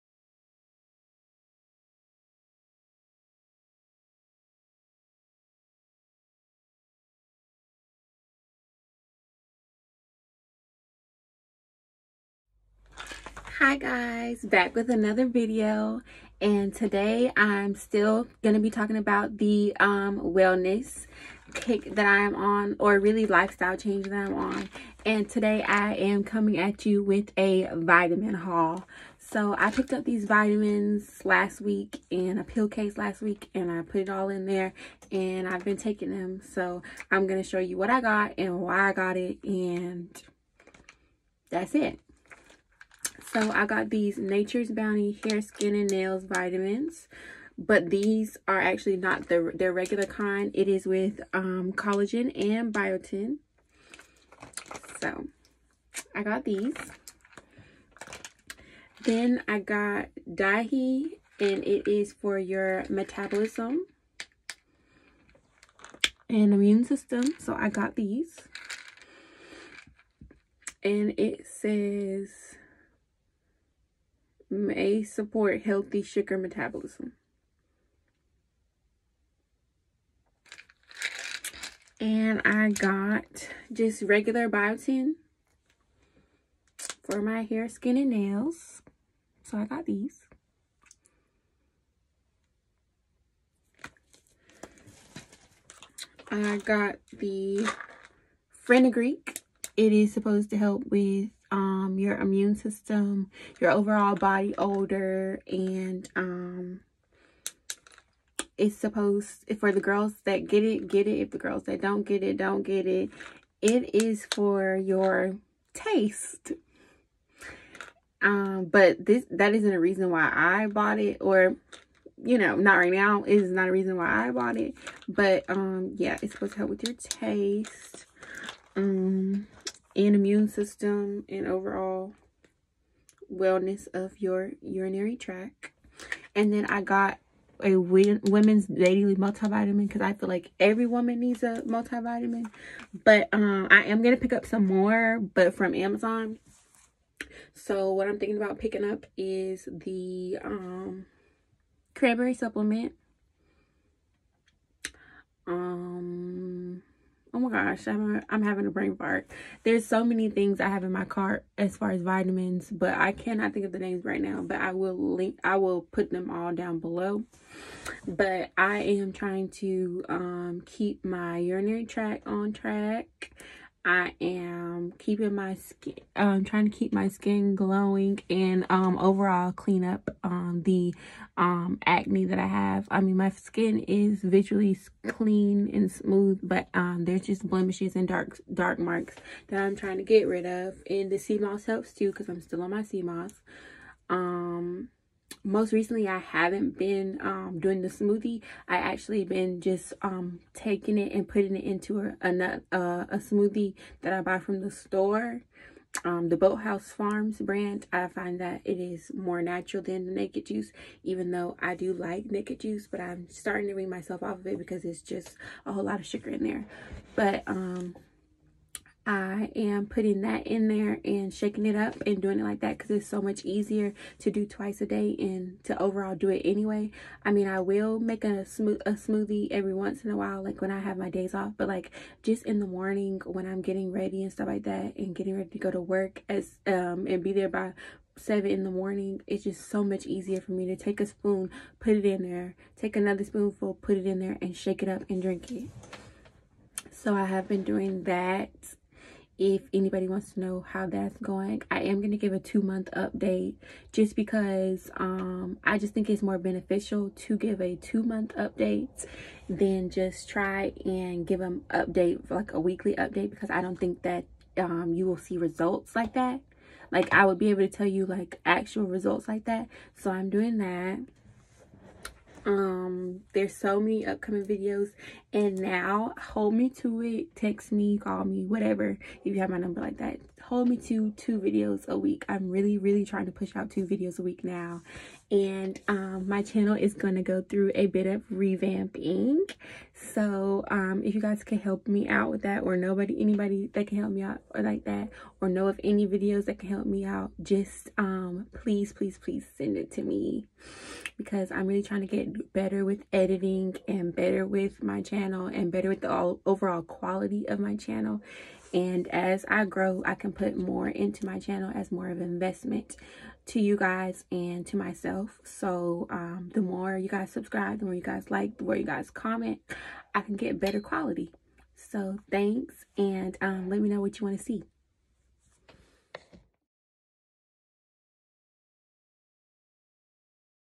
Vitamin haul
Vitamins mentioned
Natures bounty -hair skin and nails gummies
Fenugreek
Biotin gummies
Women’s daily multi vitamin gummie
Dhea

Things I plan to get
Seamoss gummies
Cranberry vitamins
Evening prime rose oil
Apple cinder vinegar
Vitamin e